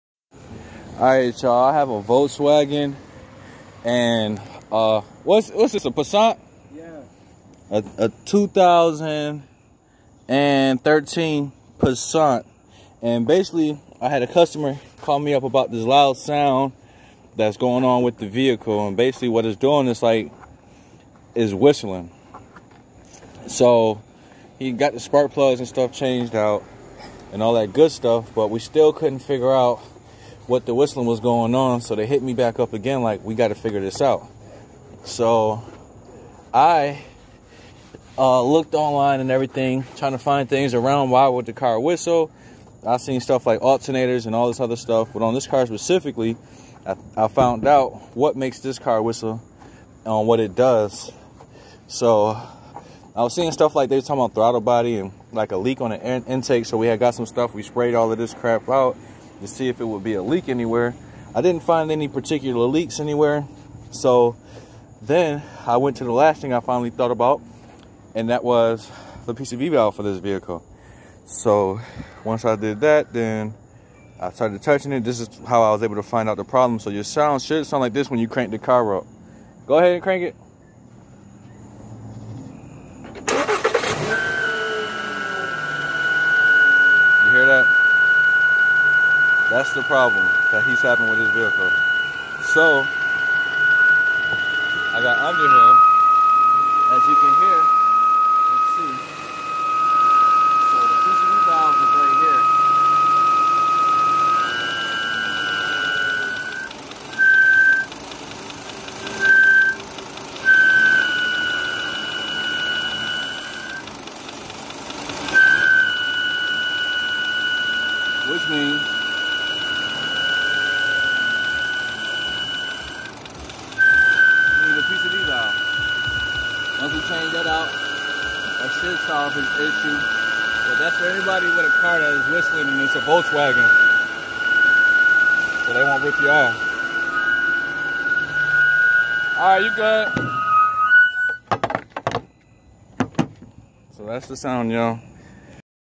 2013 VW Passat whistling .. problem found but what's wrong. Part 1..
When you have a vw that whistle while driving and it goes away then come back while driving... this video should be able to to help you solve the sound while starting and sitting...